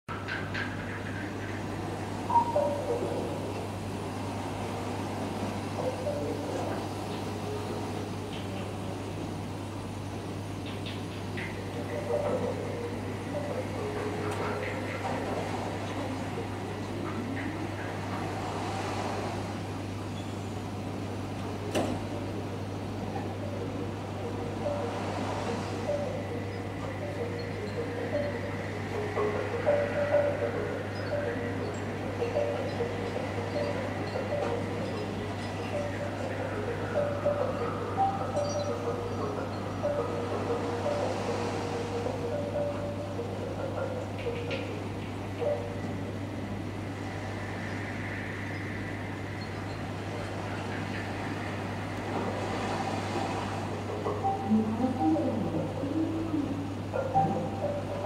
Mantis Shrimp curl up suddenly.急に丸まるシャコ。
Sumida aquarium.すみだ水族館。